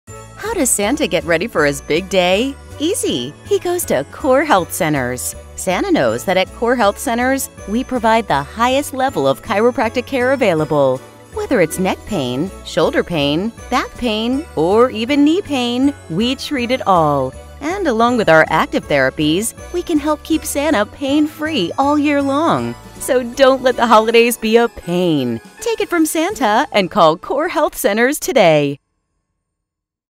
How does Santa get ready for his big day?
How does Santa get ready for his big day? Easy, He goes to CORE Health Centers! Santa knows that CORE Health Centers, provides the Highest level of Chiropractic Care available. With a combination of Active Therapies and Chiropractic Adjustments, Santa stays pain free all year long!! Don’t let the holiday’s be a pain! Call CORE Health Centers today or visit us online at gotcore.net.